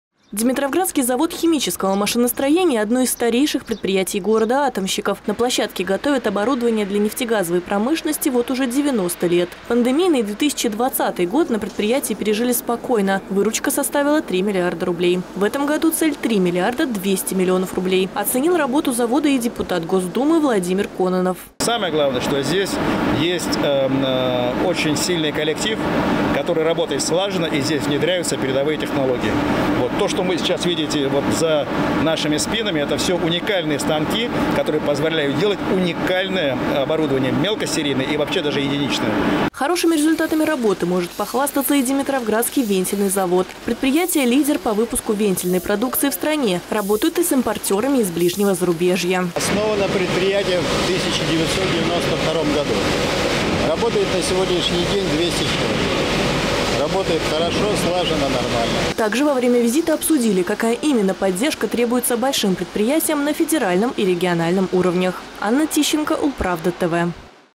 Как развиваются димитровградские промплощадки
Депутат Государственной Думы Владимир Кононов посетил одни из важнейших промплощадок второго по величине города Ульяновской области. «Димитровградхиммаш» и Димитровградский вентильный завод показывают хорошие темпы производства и выпускают востребованную продукцию для российской промышленности.